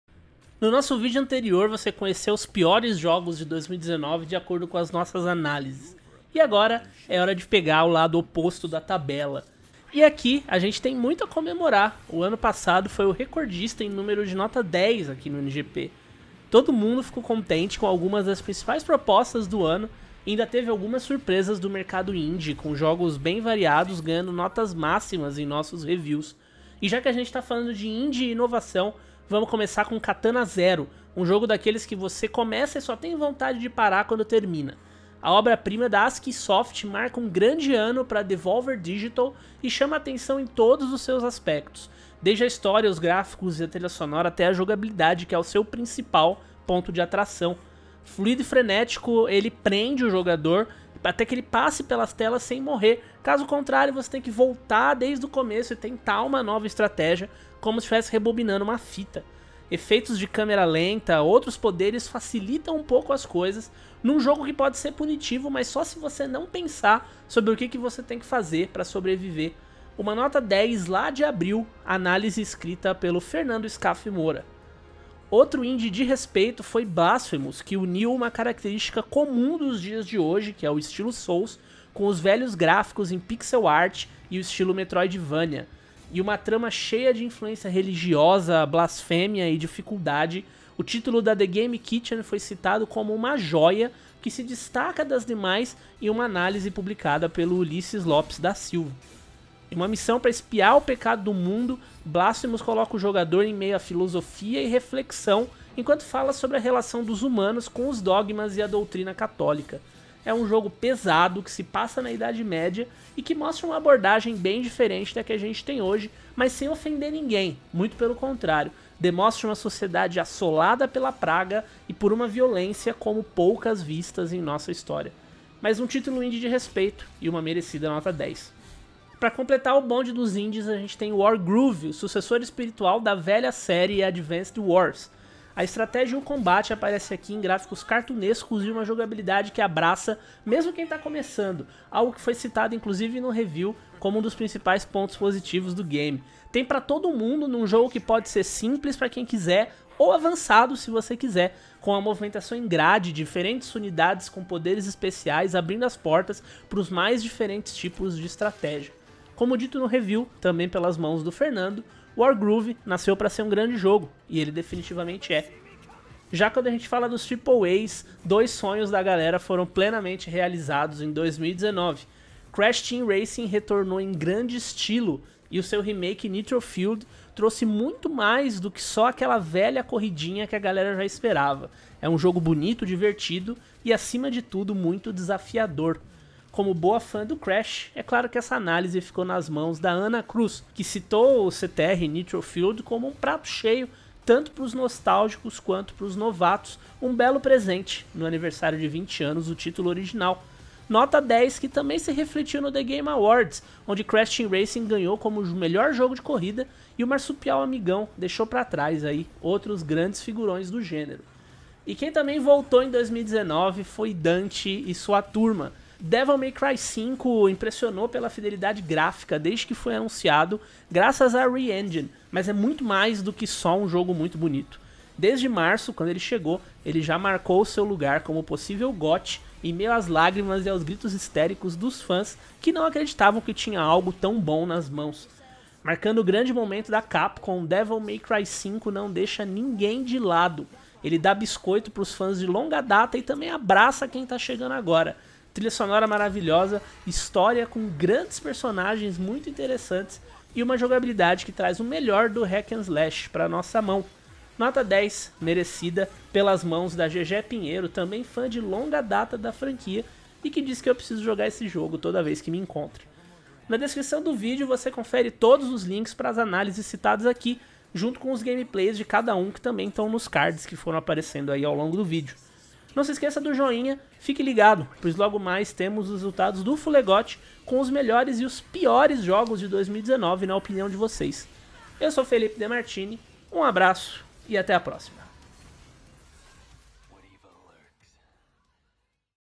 As melhores notas de 2019 no NGP
O New Game Plus bateu recordes de notas máximas em 2019, superando a já grande marca do ano passado. No período, foram nada menos do que cinco jogos com notas 10, entre games independentes de respeito e sonhos dos fãs realizados, com propostas para ninguém botar defeito em uma época na qual muitas das plataformas disponíveis no mercado chegavam ao seu potencial máximo antes do início de uma nova geração.

Por isso, antes do resultado do FuleGOTY 2019, que sai nesta semana e reúne os melhores games do ano na opinião de vocês, vamos para mais uma lista, desta vez, com os títulos considerados como ótimos pela nossa equipe. É, junto com a sequência de games que podem ter passado despercebidos, uma boa indicação para você que está procurando coisa boa para jogar neste ano que começa.

Faça uma doação para o NGP ou considere se tornar um patrocinador para ajudar o site e o canal a continuarem crescendo!